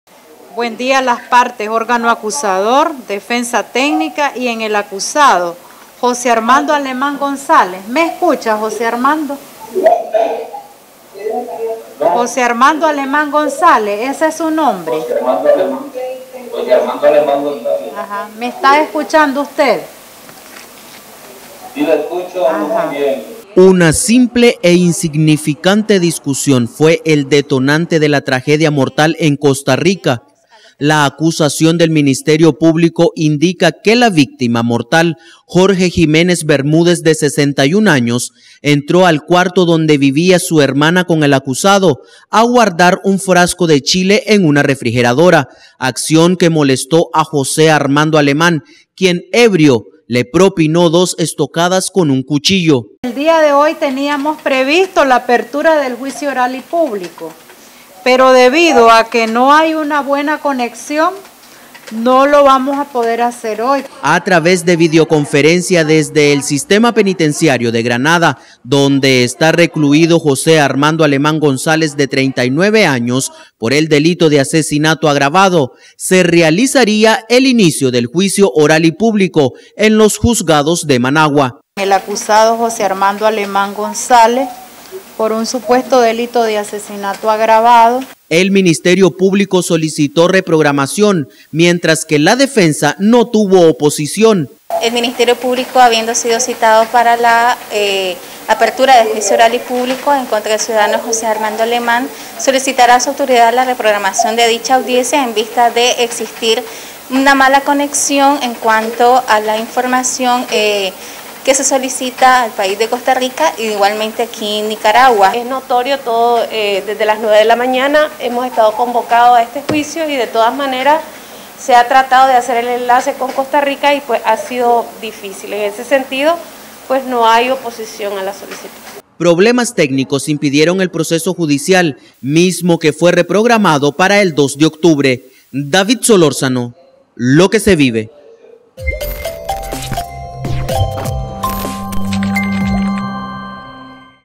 Realizan juicio virtual contra nicaragüense señalado de asesinato
El Ministerio Publico desarrolló este jueves un juicio virtual contra José Armando Alemán, señalado de asesinar a su cuñado en Costa Rica, sin embargo, por problemas de conexión, el proceso judicial fue reprogramado para el 2 de octubre.